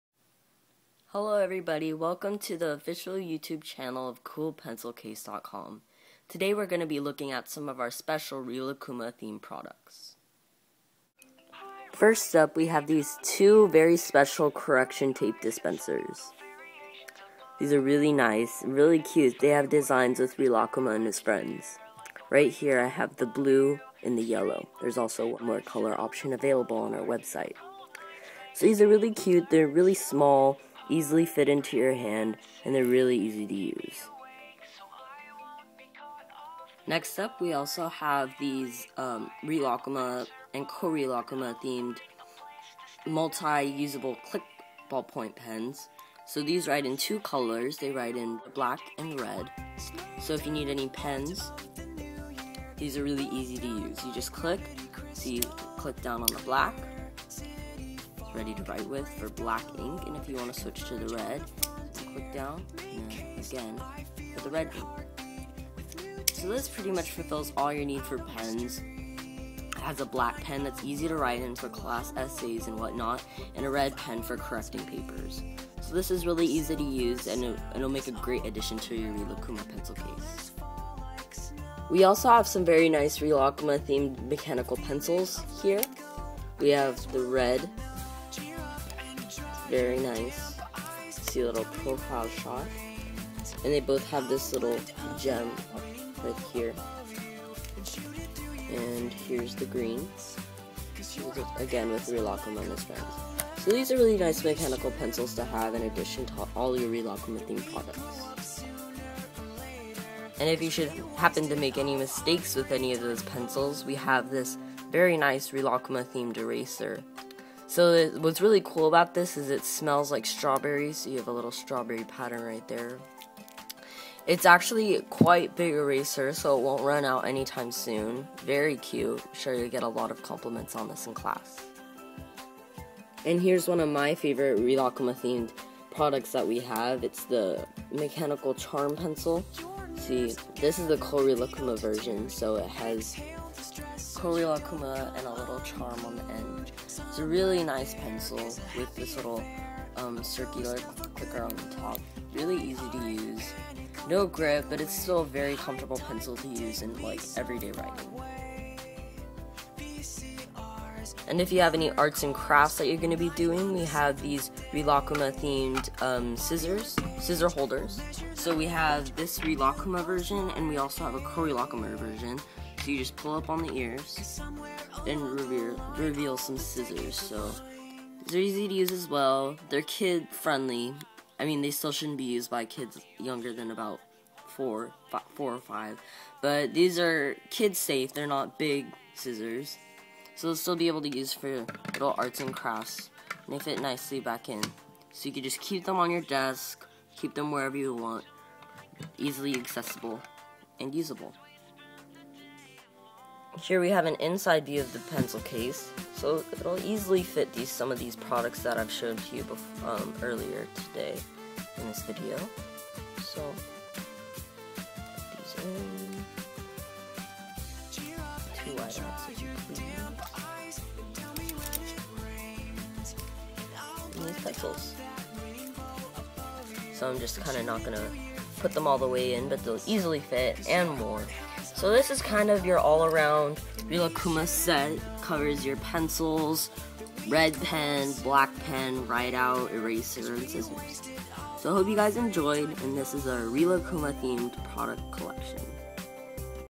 Rilakkuma Products
A video showcasing some very special Rilakkuma products from CoolPencilCase.com.